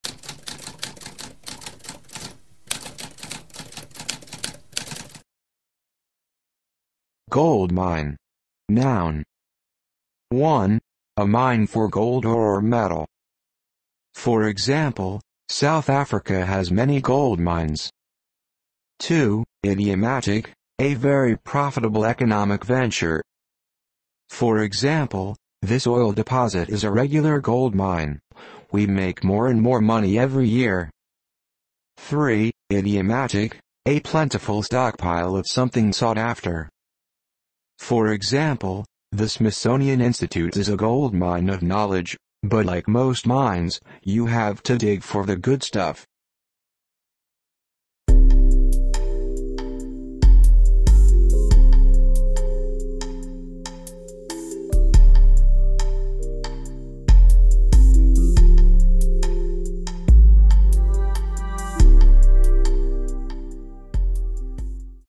What does gold mine mean?
A spoken definition of gold mine.

Intro Sound:
Typewriter - Tamskp